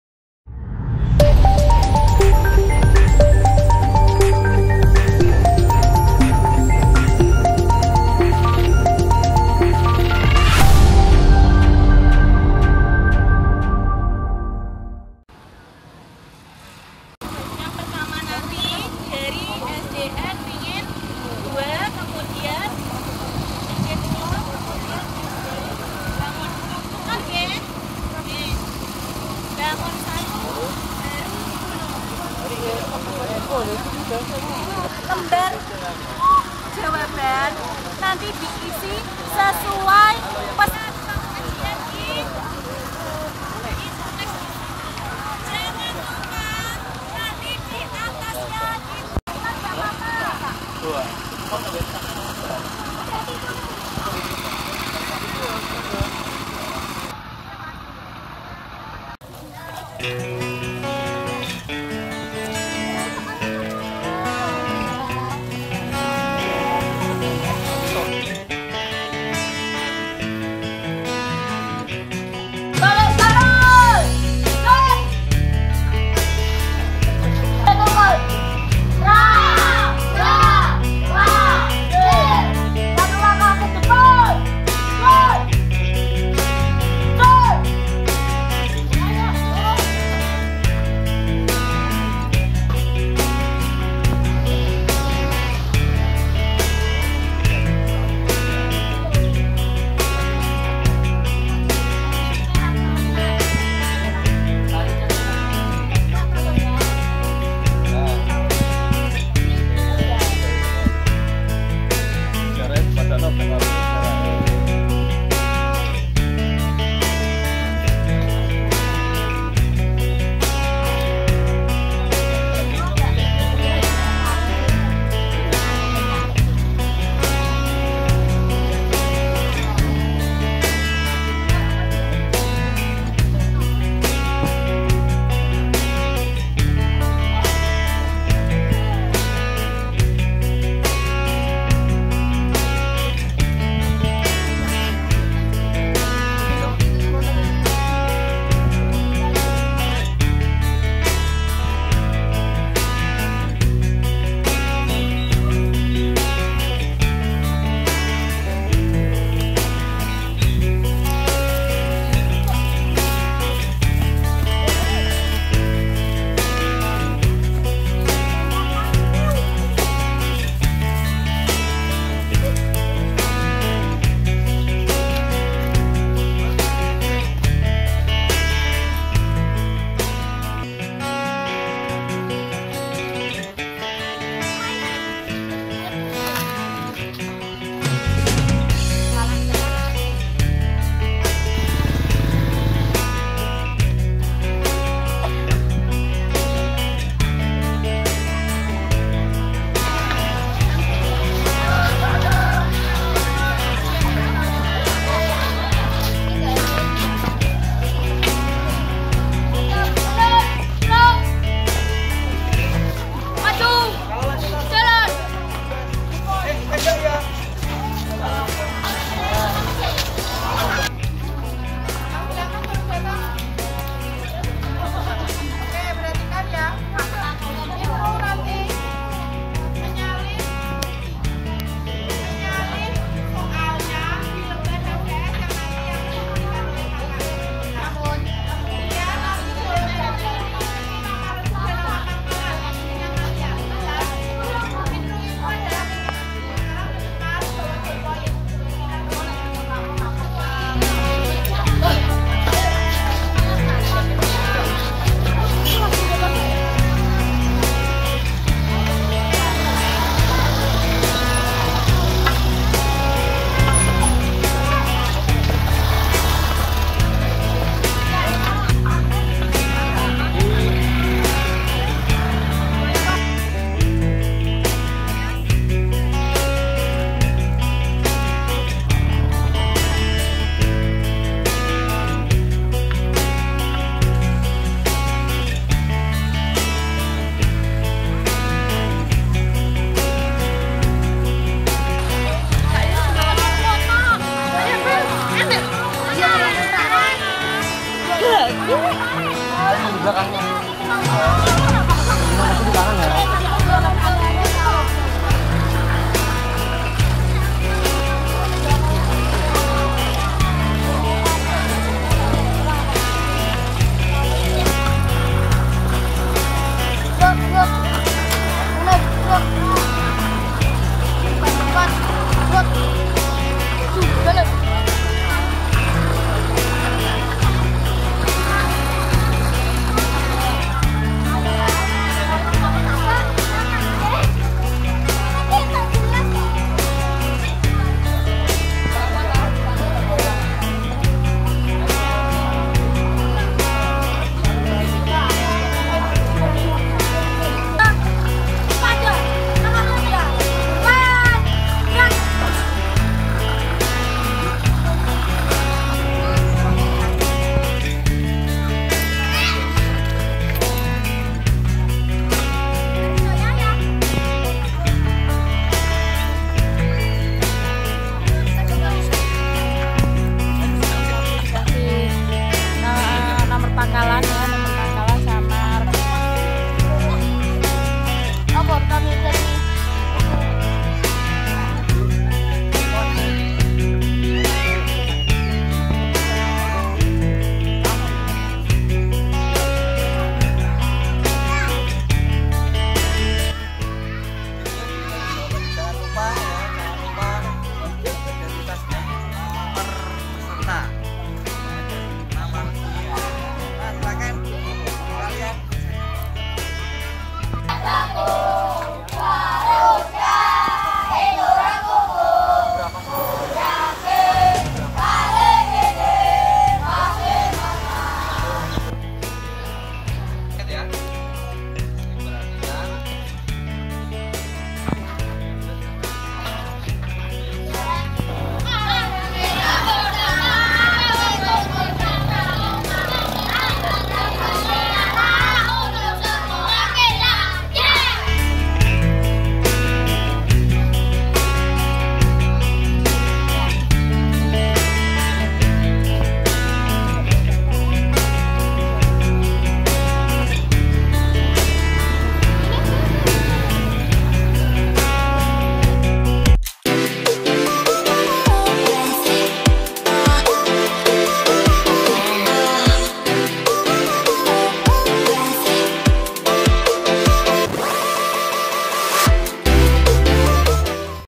Penjelajahan Kemah Jambore IX Ranting Pamotan
Penjelajahan dalam rangka meningkatkan scouting skill dan lebih dekat dengan alam dalam jambore IX Kwartir Ranting Pamotan.